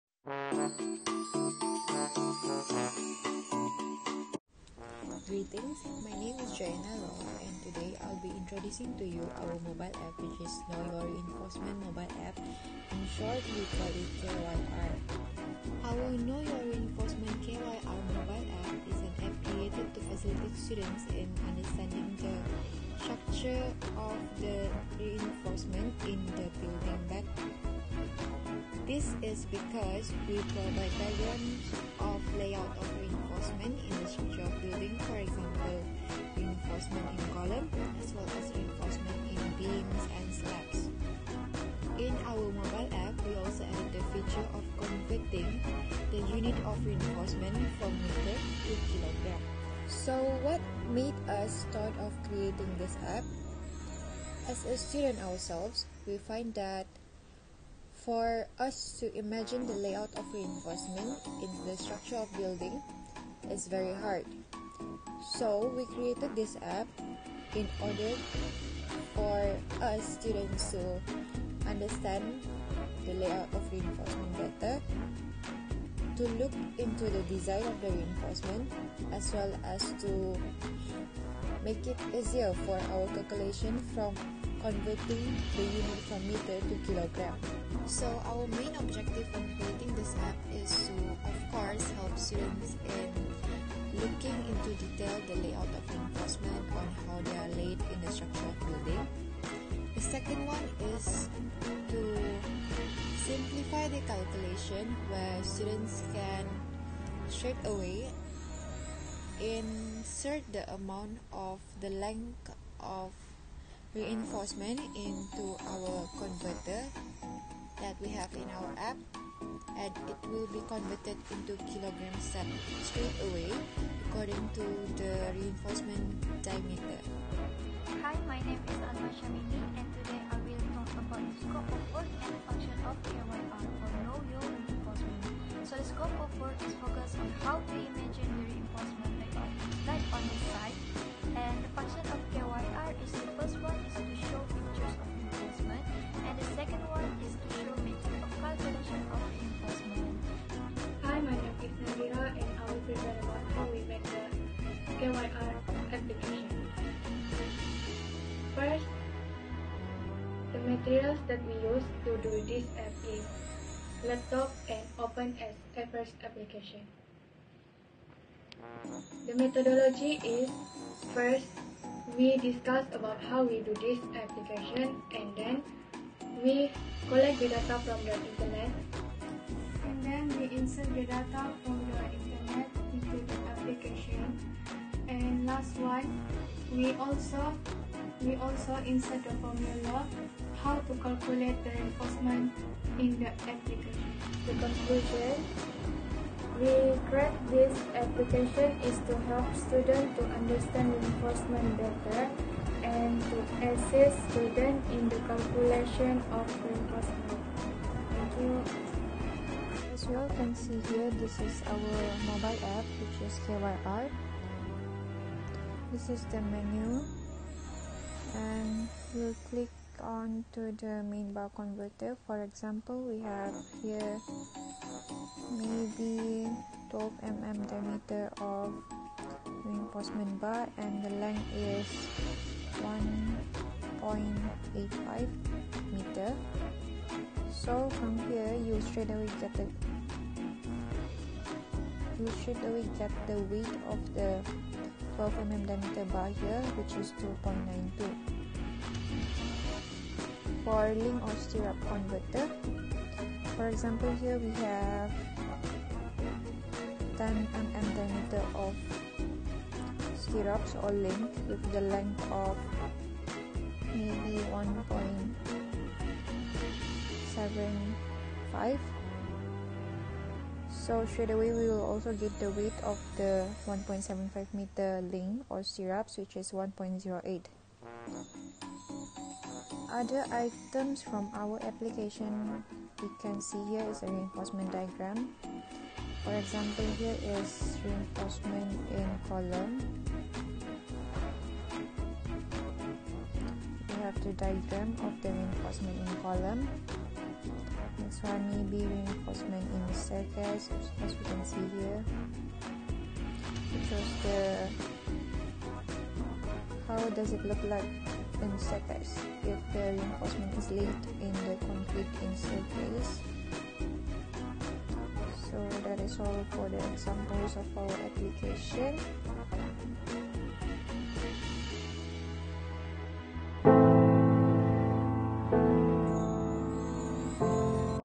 BI09: Know Your Reinforcement (KYR) App
To create an application that can help students in understanding and look into details the reinforcement in construction and to simplify converting the length of the reinforcement bar into weight.